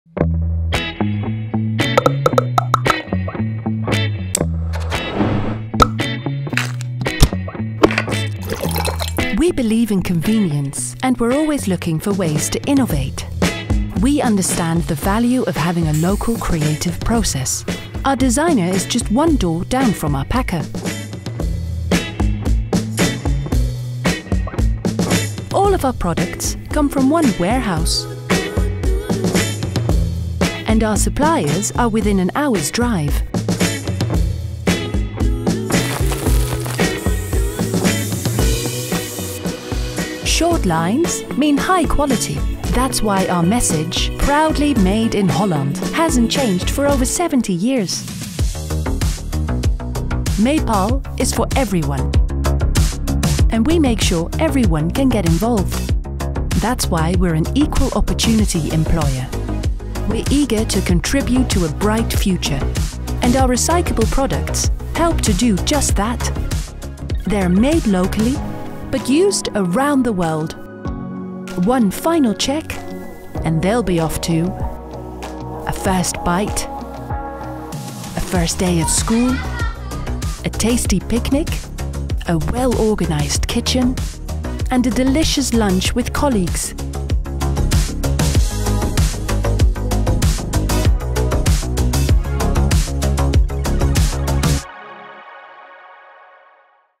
Hello, we are Mepal.
For over 70 years, we at Mepal have been developing solutions that make life easier and more carefree. We are there for the everyday small moments in life and for the special big occasions. On the first day of school, during a nice lunch with colleagues or a relaxing family holiday.  We are there for everybody, every day.

Take a look behind the scenes and discover our story.